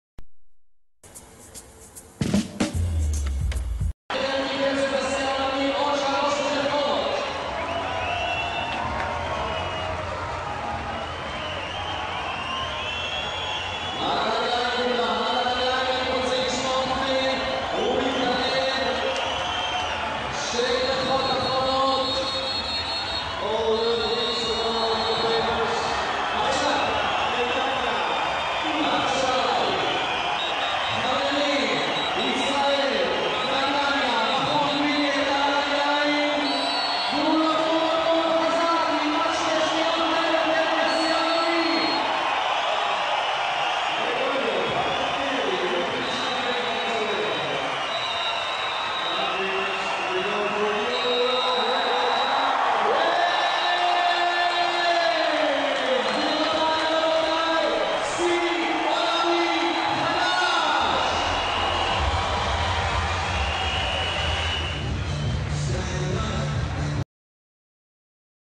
Gregorio Paltrinieri oro e record del mondo nei 1500 stile libero Europei vasca corta
Gregorio Paltrinieri oro e "record" del mondo nei 1500 stile libero Europei vasca corta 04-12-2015